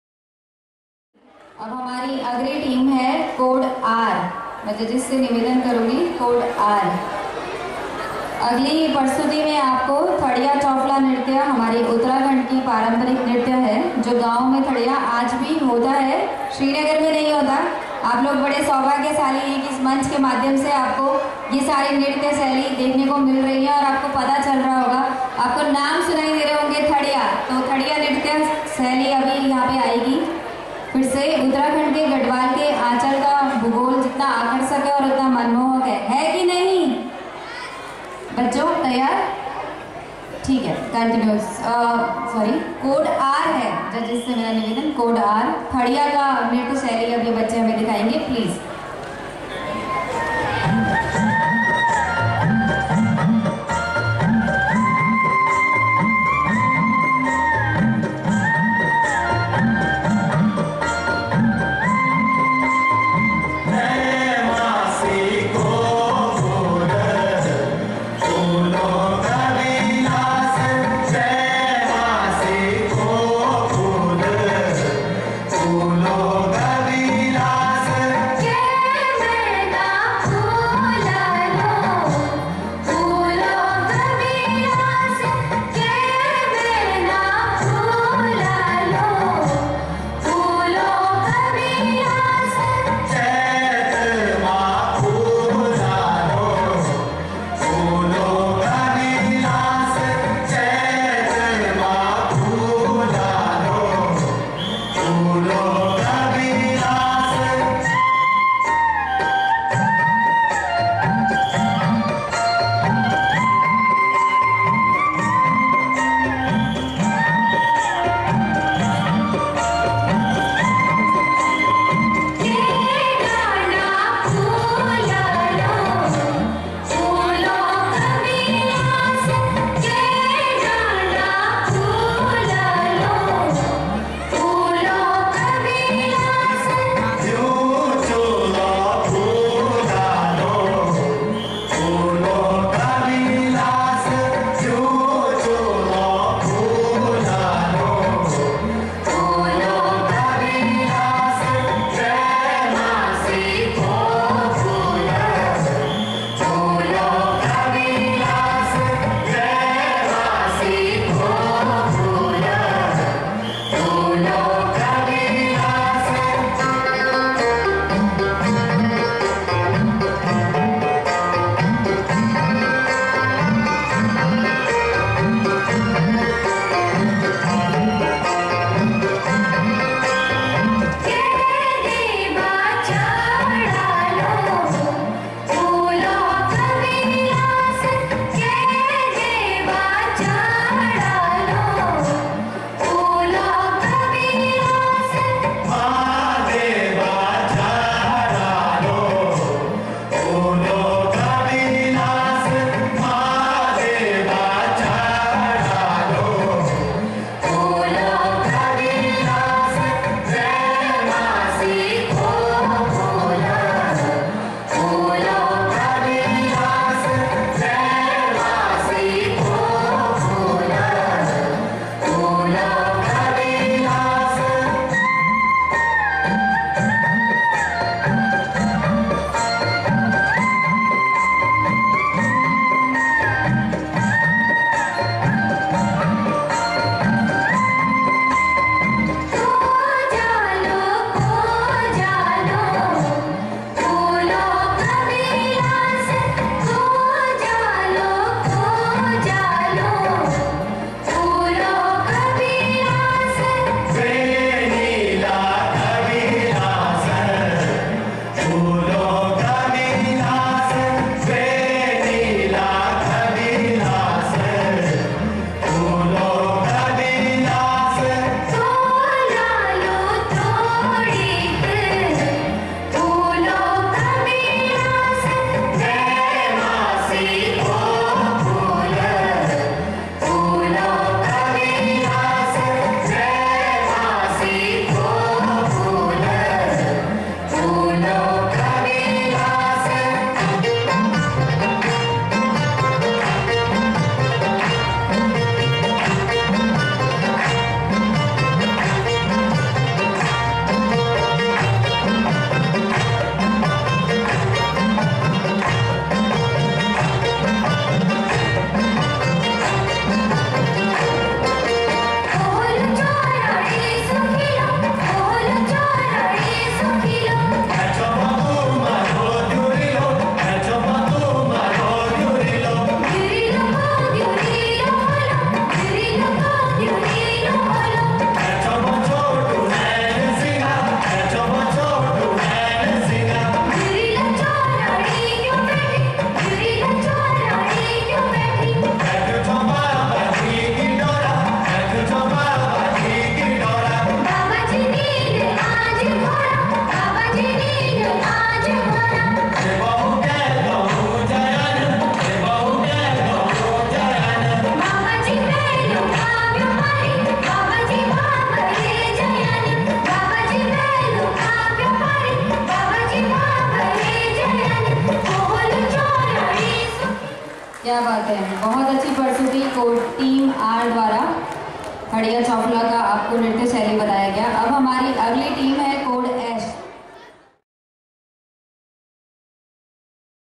Juniors Code R Thadiya Chauffla Dance in Baikunth Chaturdarshi Mela 2023 Srinagar Garhwal
Watch the Dance of Junior students of school code R in the cultural programs of Baikunth Chaturdashi Mela 2023 held in Srinagar Garhwal... they performed a very beautiful Thadiya and Chauffla dance...watch the dance of the students and appreciate their hard work by liking and commenting on their videos. Raj.